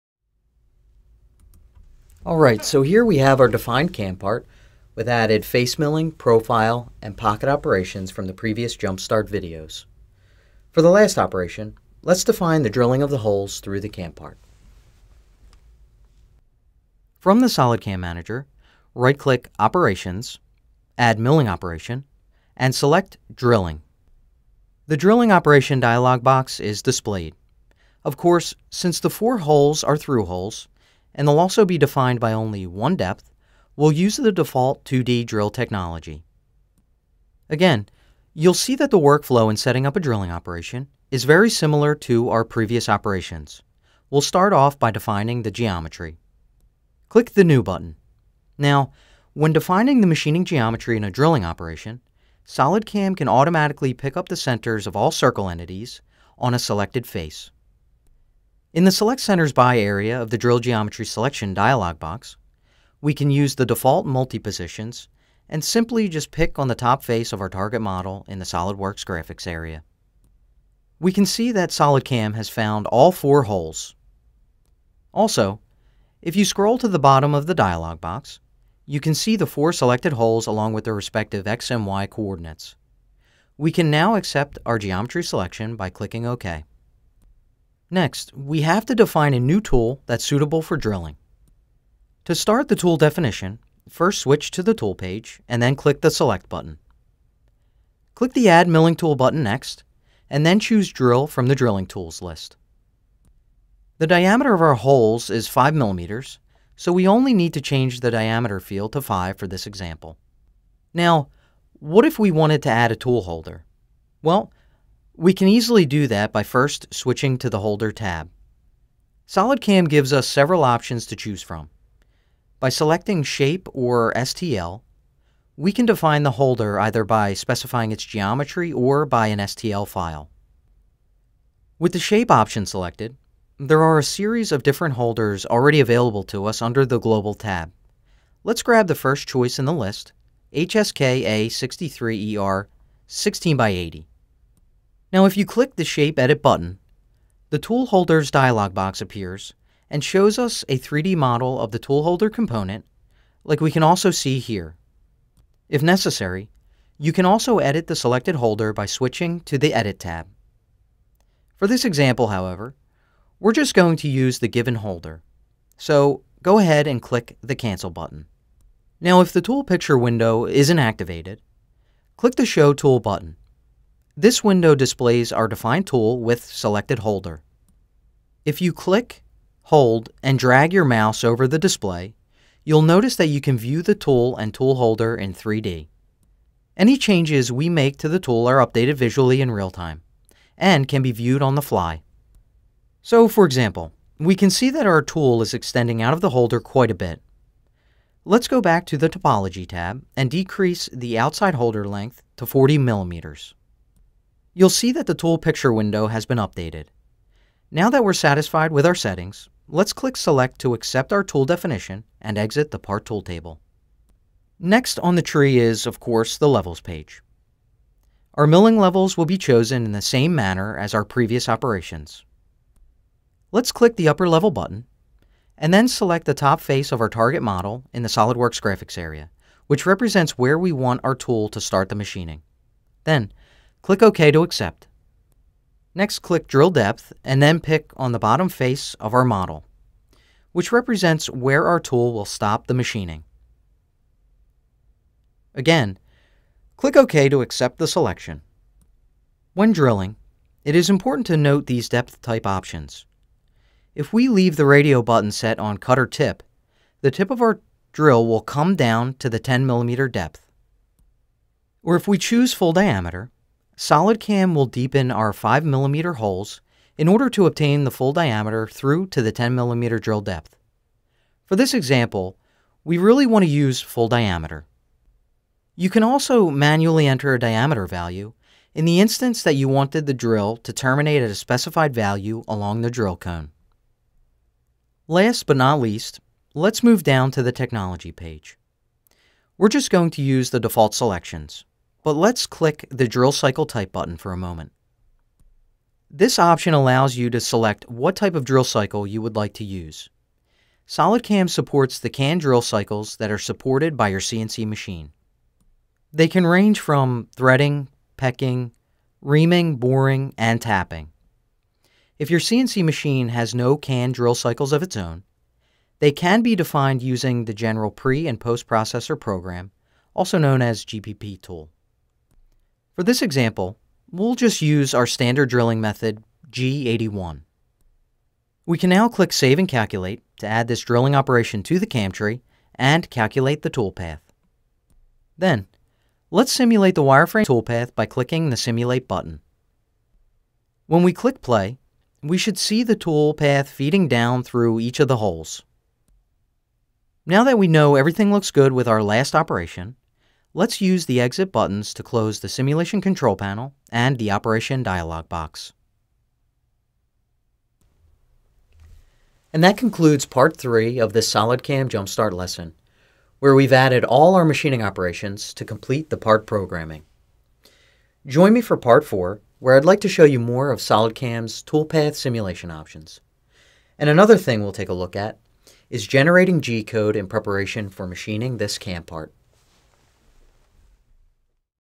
SolidCAM Jumpstart - Add a Drilling operation
This is the sixth recording of the SolidCAM Jumpstart Series, where a simple cover is programmed using some of SolidCAM's 2.5D Milling technologies. In this video, a Drilling operation is added to the CAM-Part to complete the part programming. For the Geometry, the centers of all circle entities are automatically found and defined as drill positions. A drilling tool is defined and choosing a tool holder is also shown. The Levels are again picked directly off the model, like in the previous operations. The standard drilling method of G81 is defined for the Drill cycle type. Note that SolidCAM supports the canned drill cycles that are supported by your CNC-Machine.